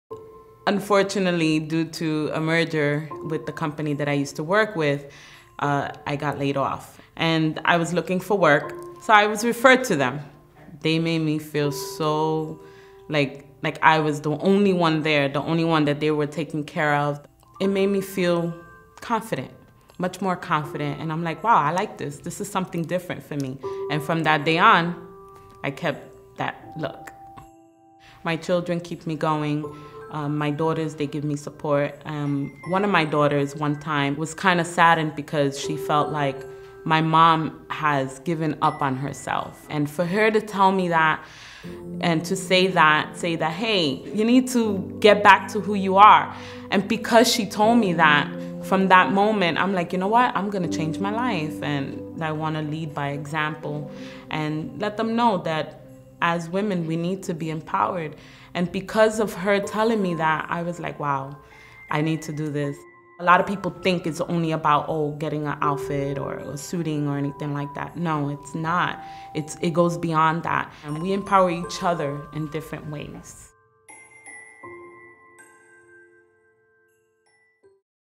Keep the Circle of Impact Going: Tania Gomez
Success Ambassador, Tania Gomez, shares her journey with Dress for Success.

Join a movement that has empowered one million women globally, changing their lives and that of their families and communities. Keep the circle of impact going and give this holiday season!